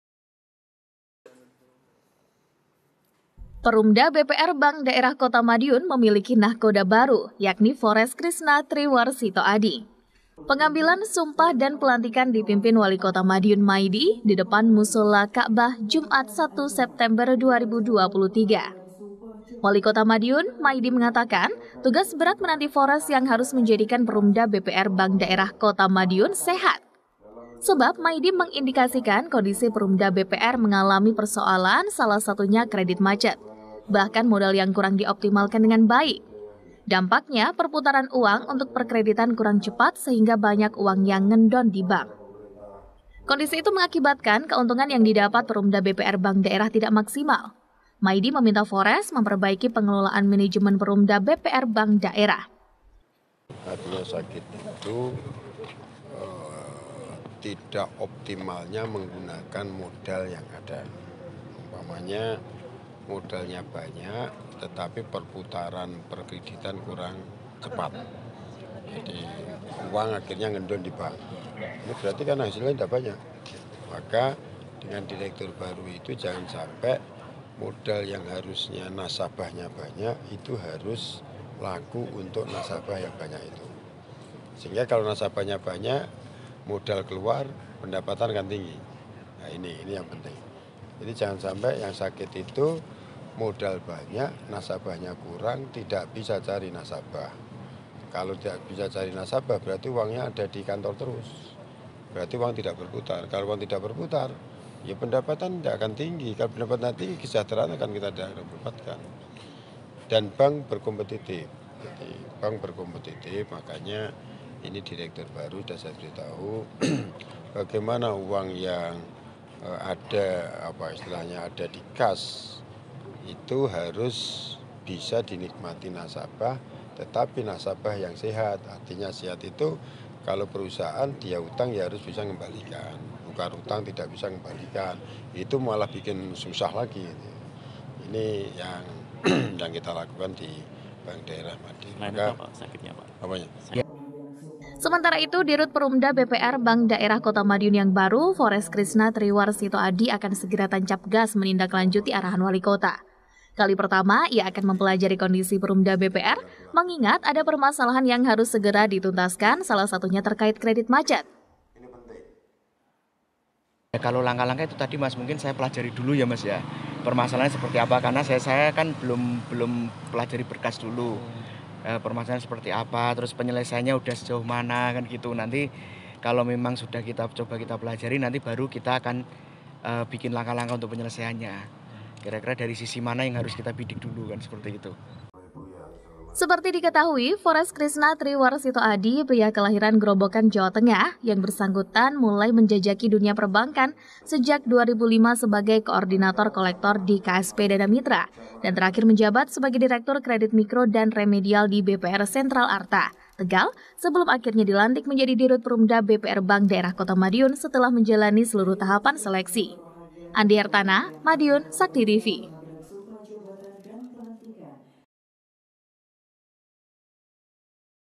MADIUN - Pelantikan Dirut Perumda BPR Kota Madiun
Perumda BPR Bank Daerah Kota Madiun memiliki nahkoda baru, yakni Forest Khrisna Tri Warsito Ady. Pengambilan sumpah dan pelantikan dipimpin Walikota Madiun, Maidi di depan Musala Ka’bah Jum’at (1/9/2023). Walikota Maidi mengatakan, tugas berat menanti forest yang harus menjadikan Perumda BPR Bank Daerah Kota Madiun sehat. Sebab Maidi mengindikasikan kondisi Perumda BPR mengalami persoalan salah satunya kredit macet, bahkan modal yang kurang dioptimalkan dengan baik.

Dampaknya perputaran uang untuk perkreditan kurang cepat sehingga banyak uang yang ngendon di bank. Kondisi itu mengakibatkan keuntungan yang didapat Perumda BPR Bank Daerah tidak maksimal. Maidi meminta forest memperbaiki pengelolaan manajemen Perumda BPR Bank Daerah.

Saksikan Info Berita Terbaru Seputar Karesidenan Madiun & Sekitarnya:

CONTACT PERSON MARKETING :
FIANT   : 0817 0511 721
LINDUNG : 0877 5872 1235 / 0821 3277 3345